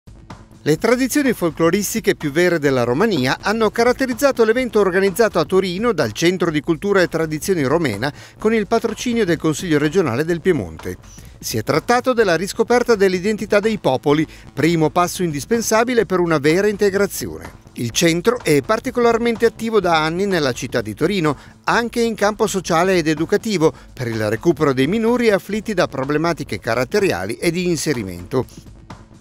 Cultura e tradizioni romene
Le tradizioni folcloristiche più vere della Romania hanno caratterizzato l'evento organizzato a Torino dal Centro di Cultura e Tradizioni Romena, con il patrocinio del Consiglio regionale del Piemonte. Si è trattato della riscoperta dell'identità dei popoli, primo passo indispensabile per una vera integrazione. Il Centro è particolarmente attivo da anni nella città di Torino anche in campo sociale ed educativo per il recupero dei minori afflitti da problematiche caratteriali e di inserimento.